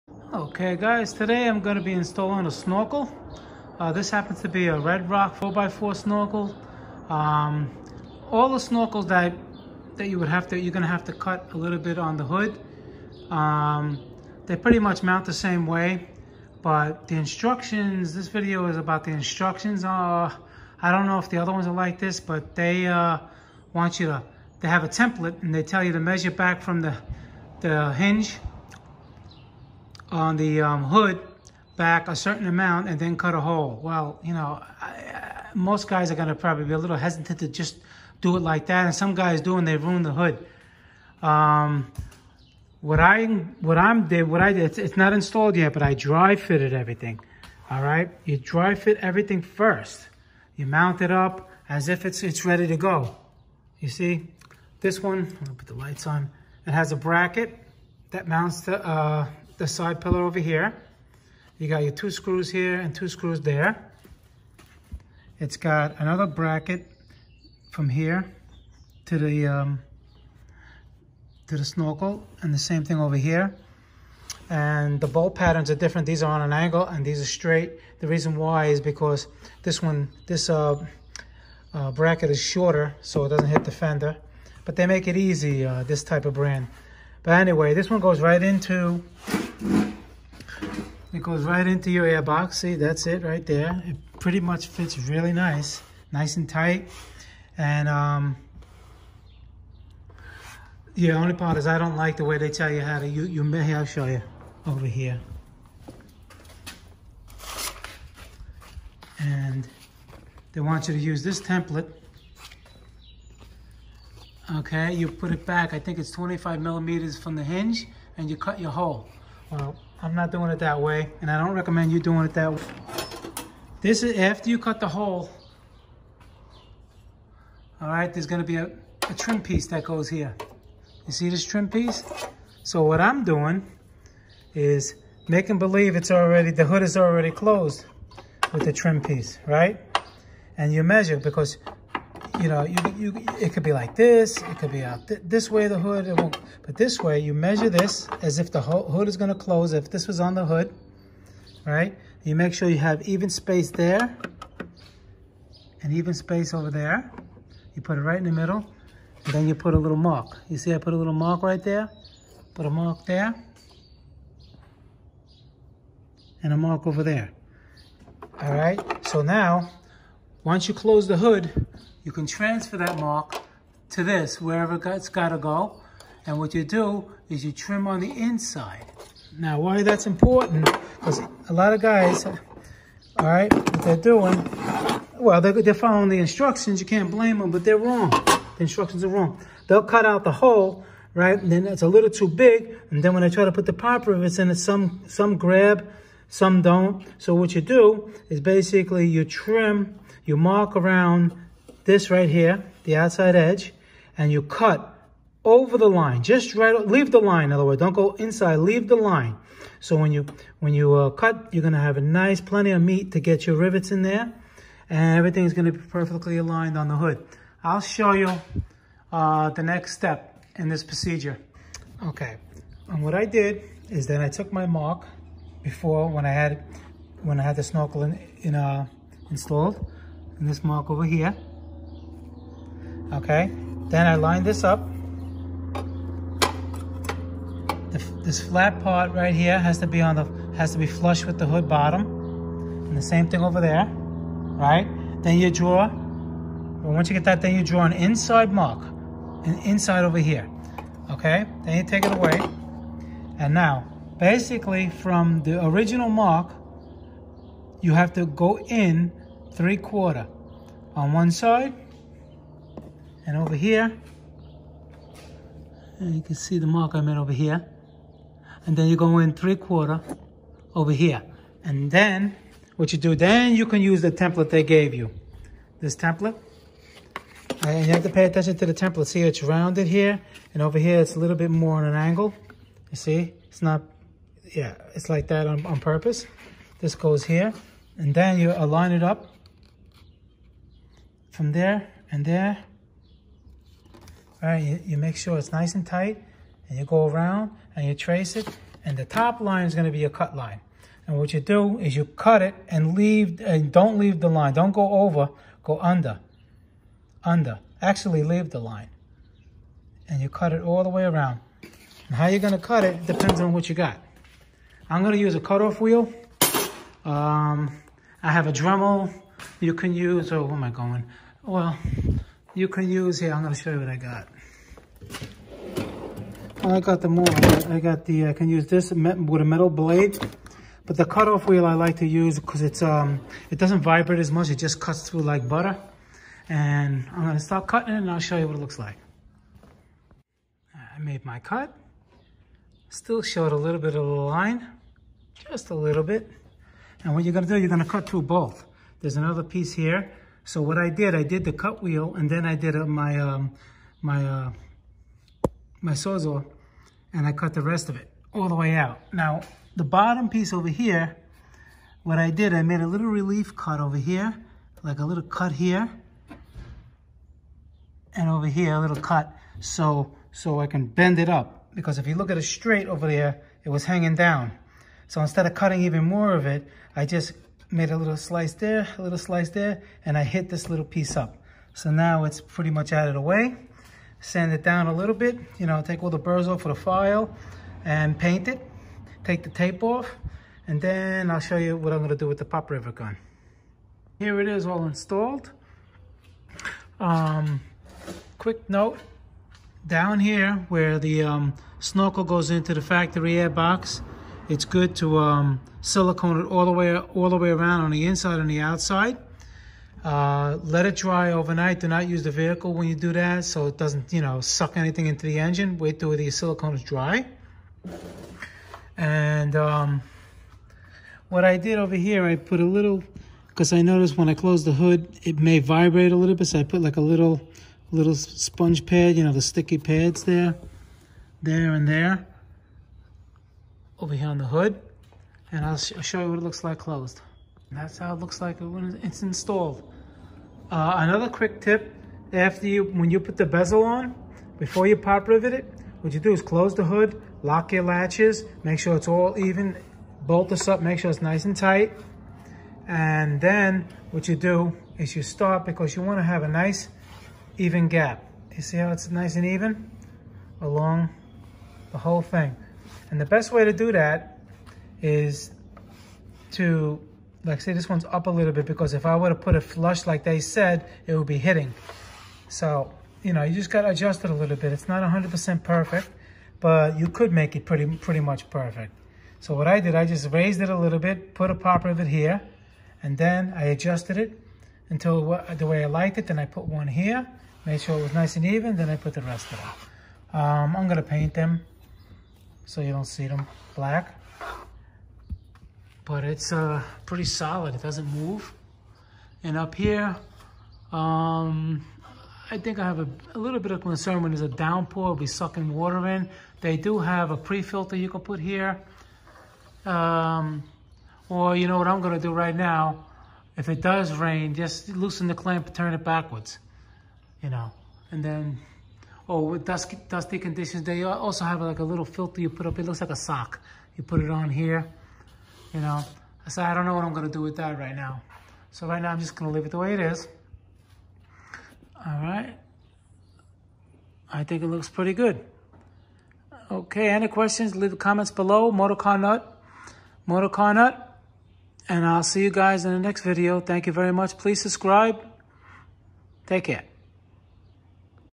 RedRock Snorkel Review & Install on Jeep Wrangler JK 2007-2011 - Follow My Instruction
Instructions that come with RedRock cold air intake snorkel a little vague and inaccurate especially when it comes to cutting your hood. Follow my instruction instead.

⁉️Questions?
I’ll have tons of videos on all kinds of car and truck repairs in the future.